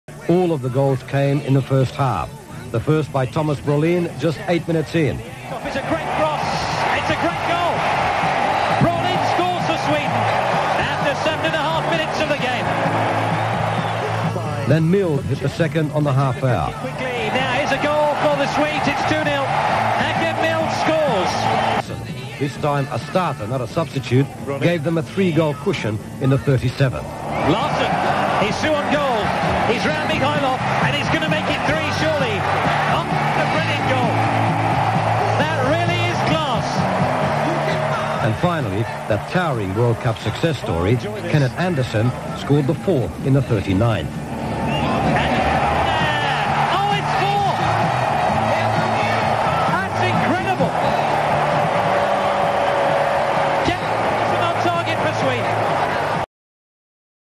OnThisDay : Sweden vs Bulgaria - 3rd Place game - USA 1994
The goals of the game for the 3rd place of the FIFA World Cup USA 1994, between Sweden and Bulgaria!